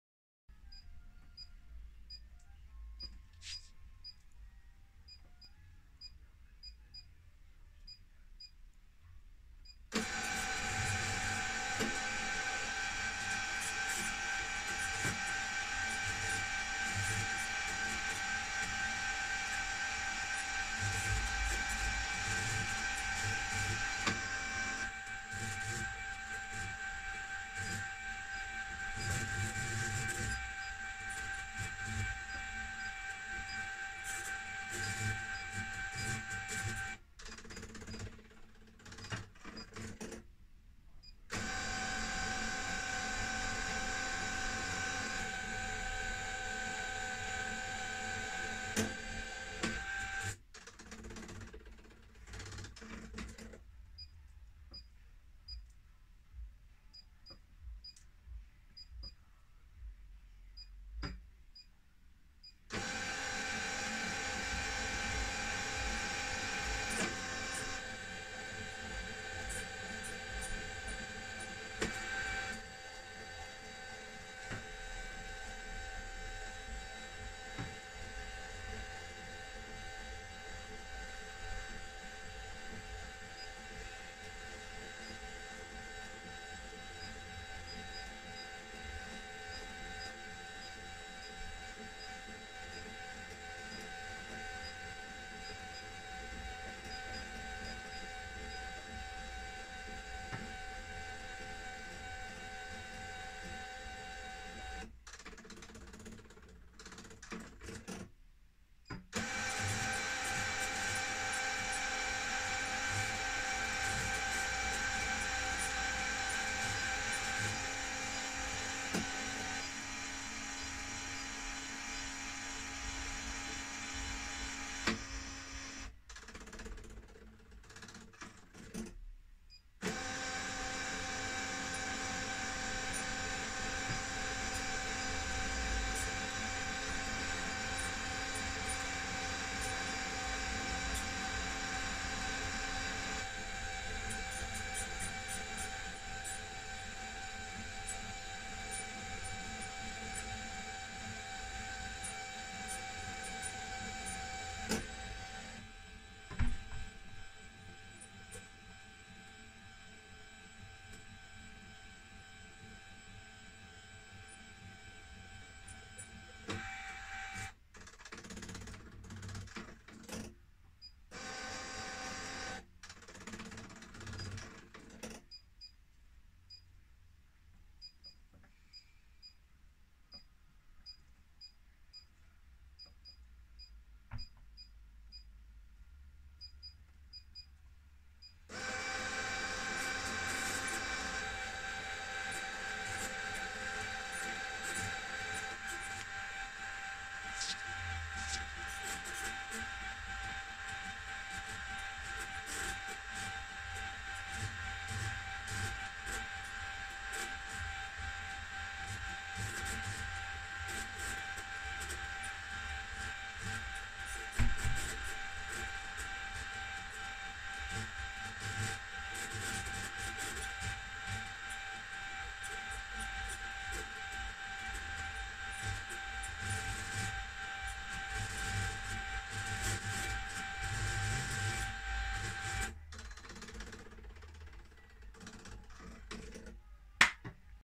Bright Tech Developments Number Blind
Spelling some famous synths and drum machines on it.